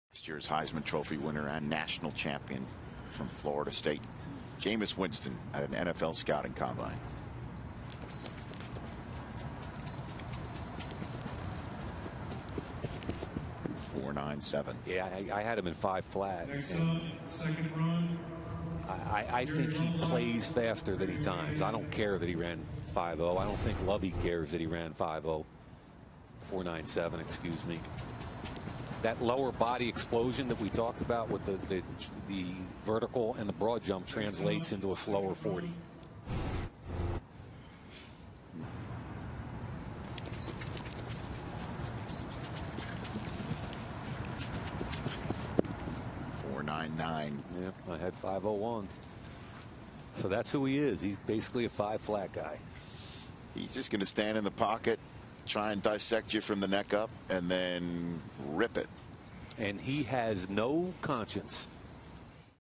Florida St. QB Jameis Winston 40 Yard Dash
Florida State University quarterback Jameis Wisnton runs a 4.97 40 yard dash time at the NFL Combine.

Visit TheSportSaga website for in-depth coverage on Florida sports!